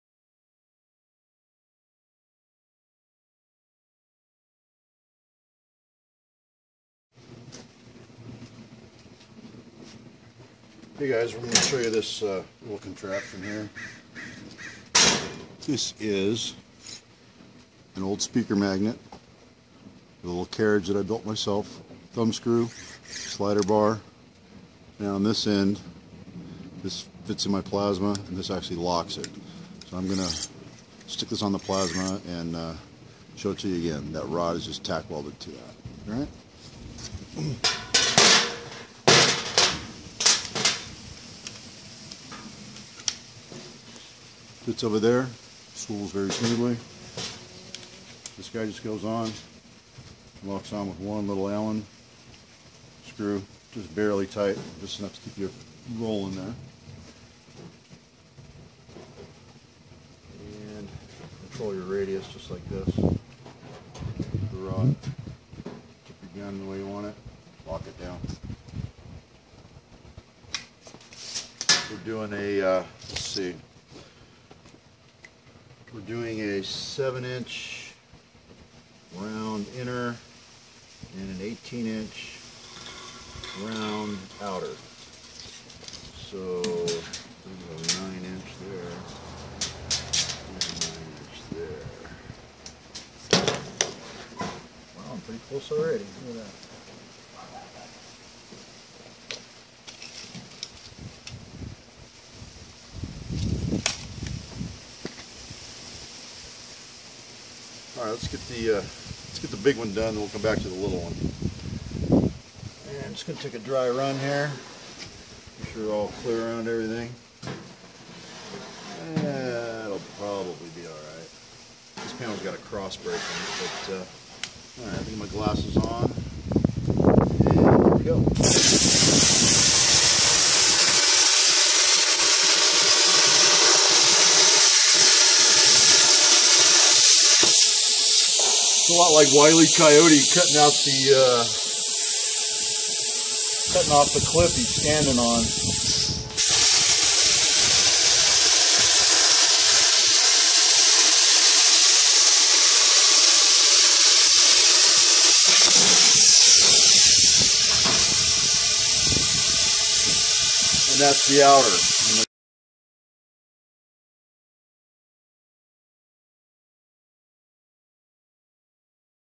Shop built plasma compass
Easy to make plasma compass, uses an old speaker magnet and some 1/4" rod